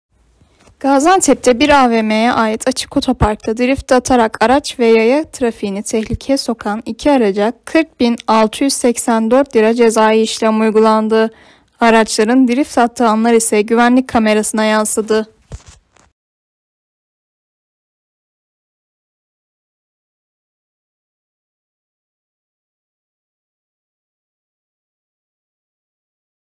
Drift atan 2 araca 40 bin lira ceza
Gaziantep’te bir AVM’ye ait açık otoparkta drift atarak araç ve yaya trafiğini tehlikeye sokan 2 araca 40 bin 684 lira cezai işlem uygulandı